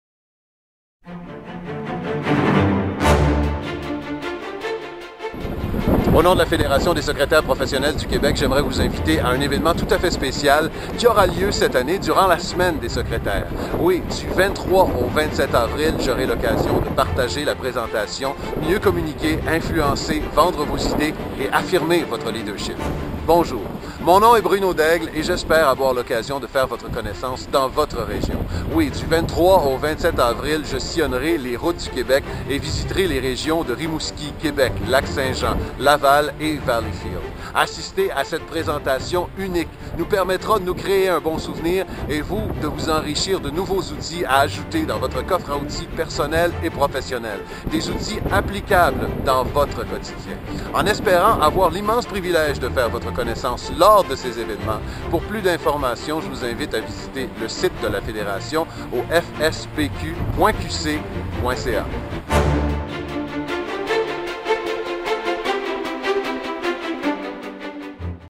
Invitation à la semaine des secrétaires ! Bruno Daigle en conference aux 4 coins du Québec !
Assistez à la conférence formation : Mieux communiquer, influencer, vendre vos idées et affirmer votre leadership // Du 23 au 27 avril - Rimouski, Québec, Lac St-Jean, Laval, Valleyfield -